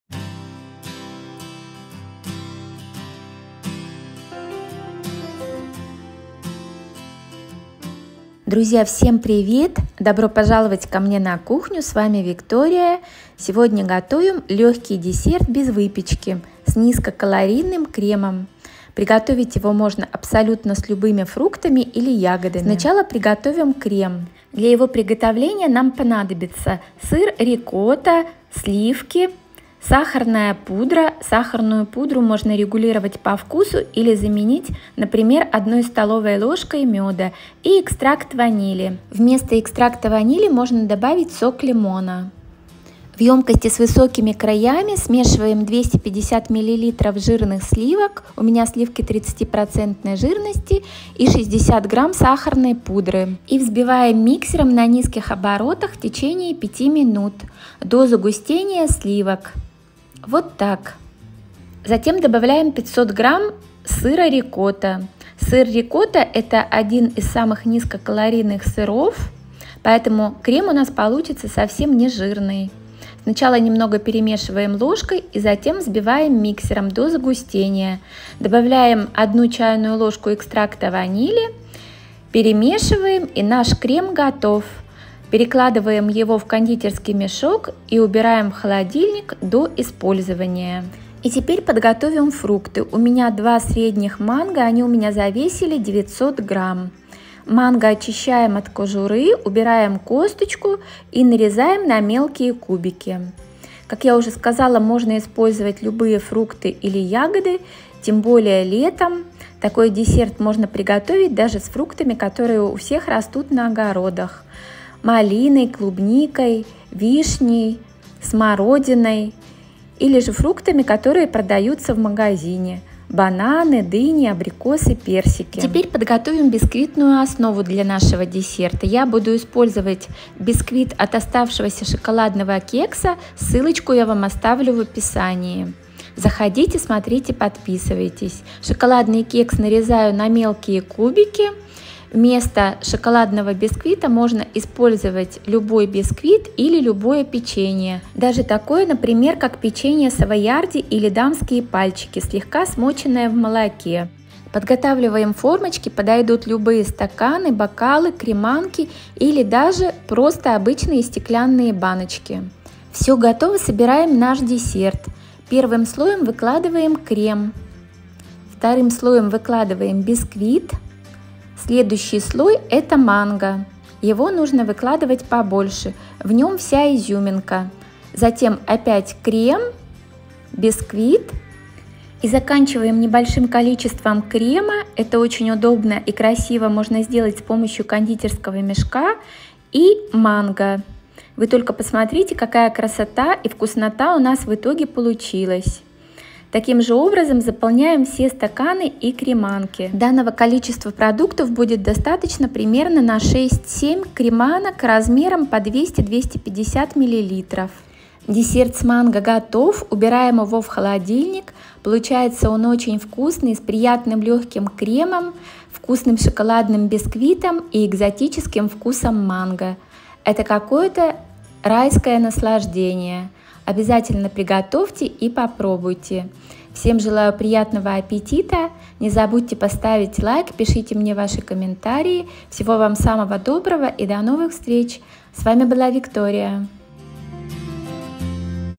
Легкий Десерт без выпечки ! Низкокалорийный Десерт с манго !
Друзья, сегодня готовим Легкий Десерт без выпечки - Низкокалорийный Десерт с манго или как его еще называют Трайфл ! Этот Нежный десерт ( ПП десерт ) прекрасно украсит ваши летние дни , ингредиенты можно менять по сезону и вкусу ! Также он прекрасно украсит любой Праздничный стол !

Ингредиенты :
Манго - 2 шт - 900 г в неочищенном виде ( Можно использовать любые фрукты и ягоды по сезону : клубника, смородина, вишня, малина, бананы, абрикосы, персики, дыня ...)
Сыр Рикотта - 500 г ( можно заменить творогом )
Сахарная пудра - 60 г ( можно меньше или без или 1 ст л меда )
Экстракт ванили - 1 ч л ( или сок лимона)